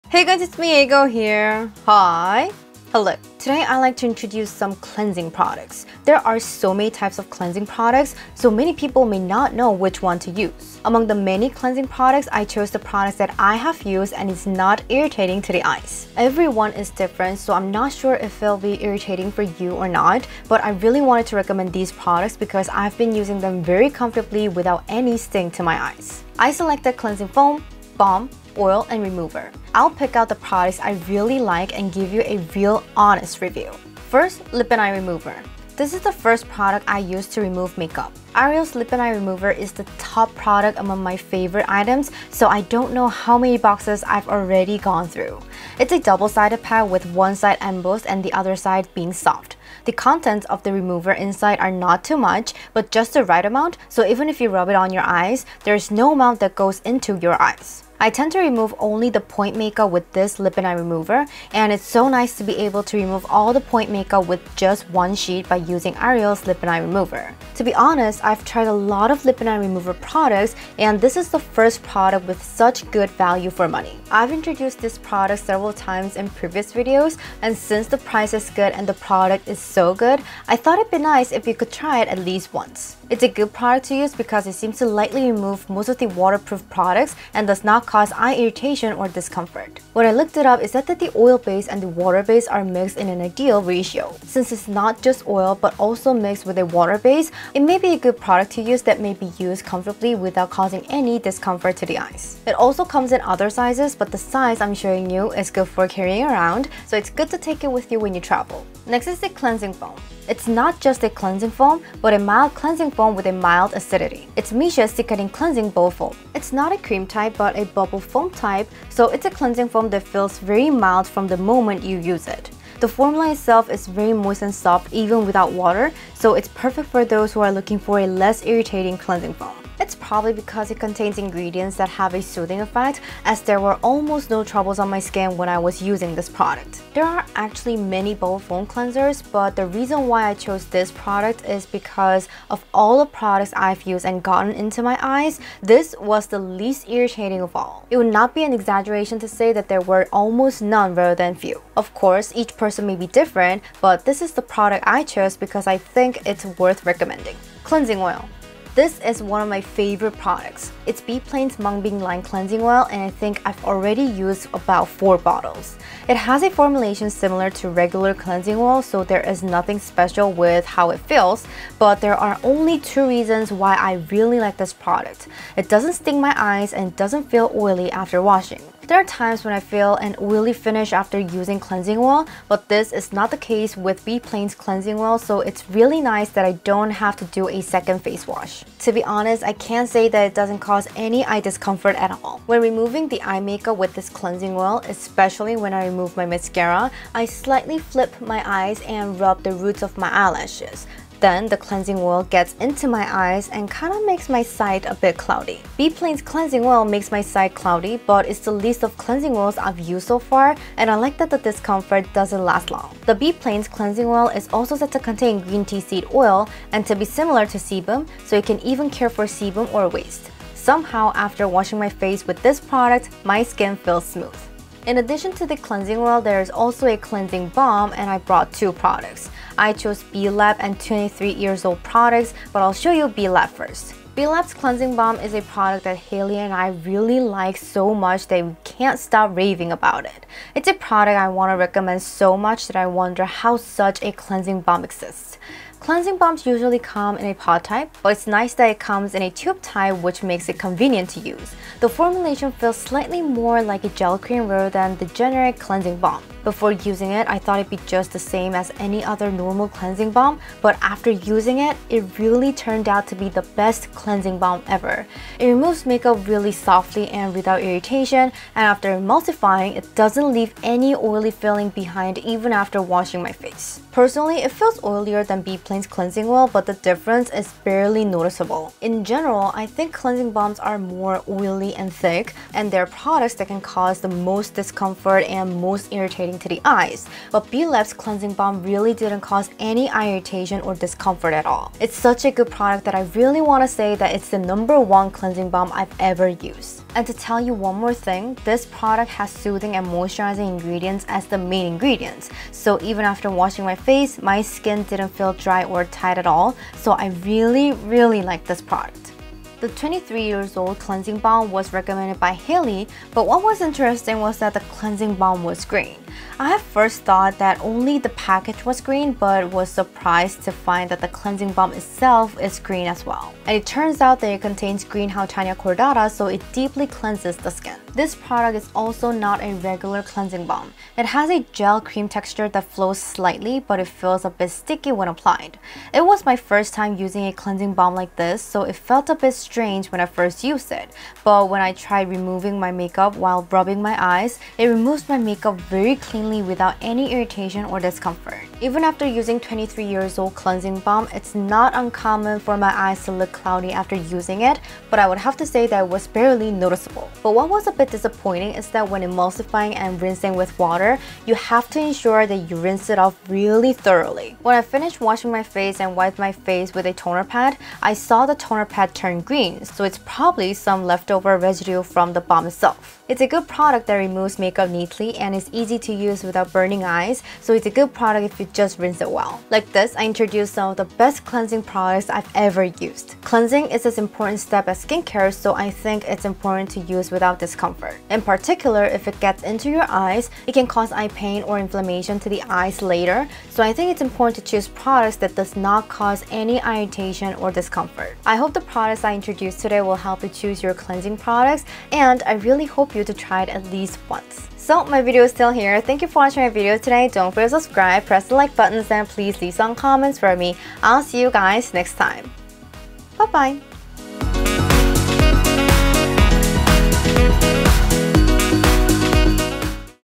BEST 5 ❤️‍🔥 Real NON EYE IRRITATING Cleanser products | Lip&Eye remover, Cleansing Foam, Oil, Balm
Hey guys, it’s me Aego!

Today, I would like to introduce some cleansing products.

Among the many cleansing products, I chose the products that I have used and is not irritating to the eye.

⏱TIME LINE⏱
00:00 - Greeting
00:26 - Cleansing products
00:34 - Arial Lip&Eye Remover
01:49 - Missha Foam Cleanser
02:38 - Beplain Cleansing Oil
03:54 - Blab Cleansing Balm
05:10 - 23yearsold Cleansing Balm
06:20 - Side thought
06:45 - Closing